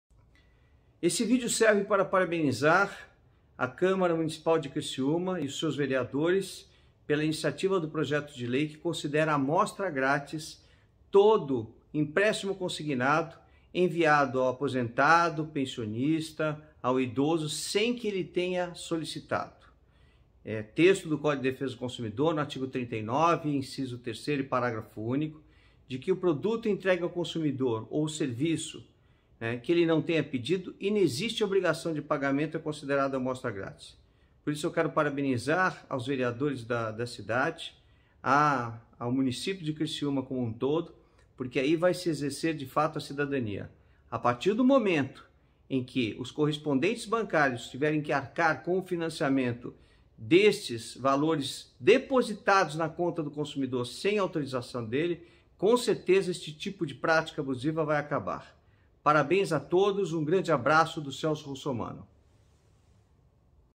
Deputado Celso Russomanno e a "amostra grátis" nos empréstimos
O deputado federal Celso Russomanno (Republicanos-SP) enviou vídeo parabenizando a aprovação do projeto que transforma em amostra grátis os empréstimos consignados feitos sem autorização em Criciúma.